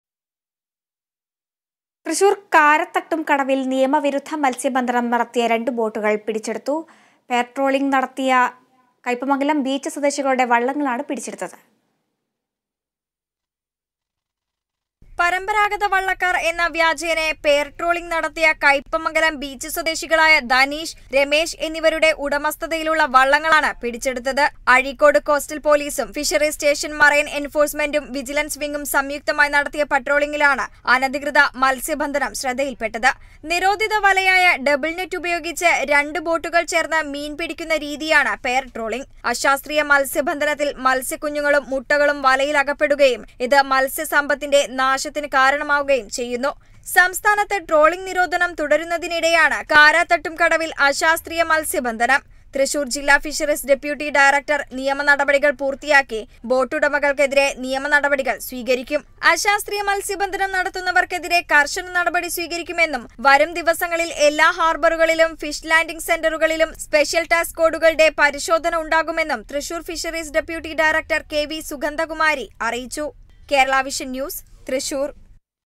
തൃശൂര്‍ കാരതട്ടും കടവില്‍ നിയമ വിരുദ്ധ മത്സ്യബന്ധനം നടത്തിയ 2 ബോട്ടുകള്‍ പിടിച്ചെടുത്തു
തൃശൂര്‍ കാരതട്ടും കടവില്‍ നിയമ വിരുദ്ധ മത്സ്യബന്ധനം നടത്തിയ 2 ബോട്ടുകള്‍ പിടിച്ചെടുത്തു. പെയര്‍ ട്രോളിങ്ങ് നടത്തിയ കൈപ്പമംഗലം ബീച്ച് സ്വദേശികളുടെ വള്ളങ്ങളാണ് പിടിച്ചെടുത്തത്

About COA